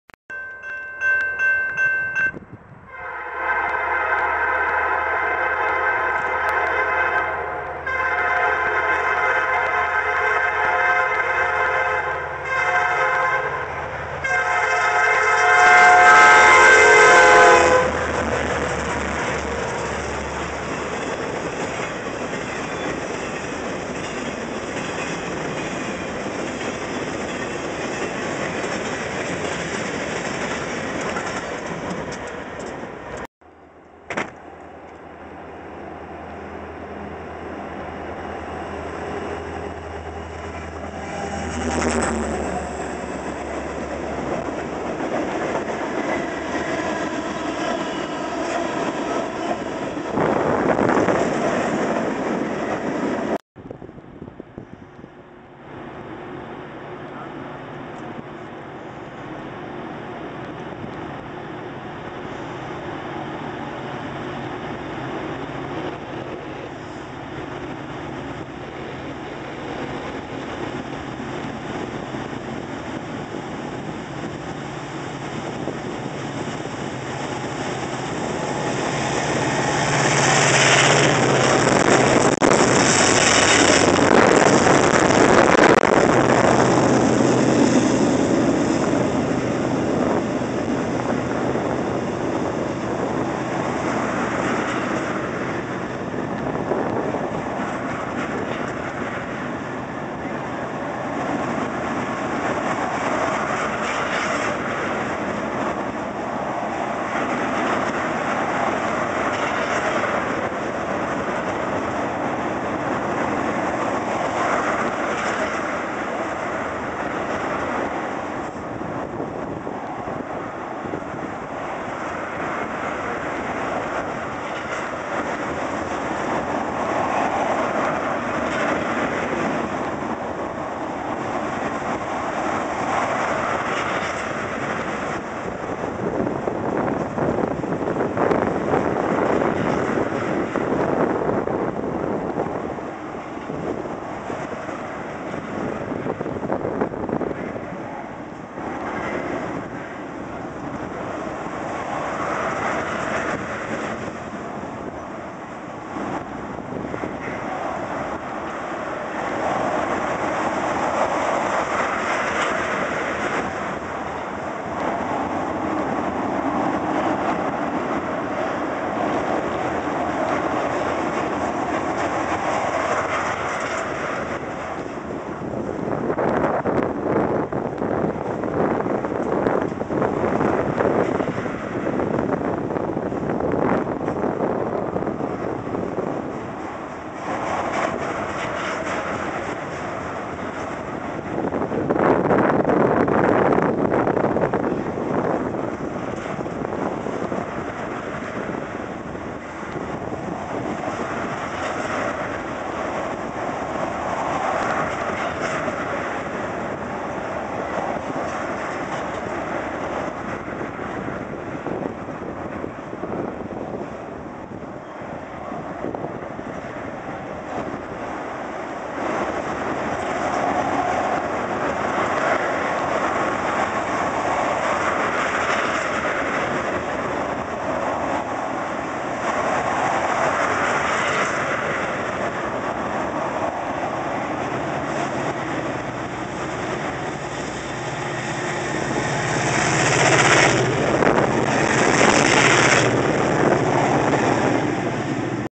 Norfolk Southern Freight
From my station inn trip, 1st train is at carneys crossing the rest are from cassandra bridge.